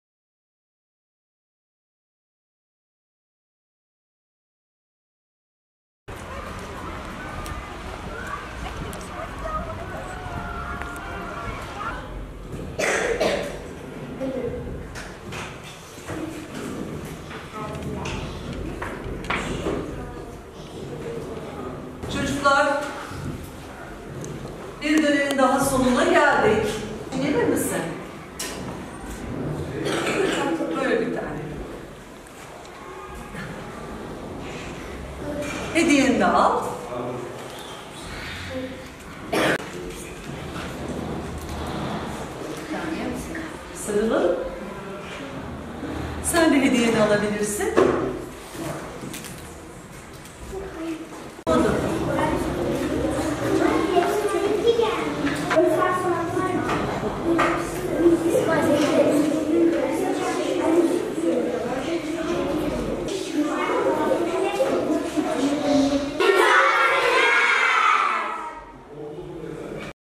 Öğrenciler karne heyecanı yaşadı
Karabük'te eğitim öğretim yılının birinci döneminin sona ermesiyle öğrenciler karne sevinci yaşadı. Şehit Mehmet Dinçel İlkokulu'nda okuyan öğrenciler karnelerini aldı.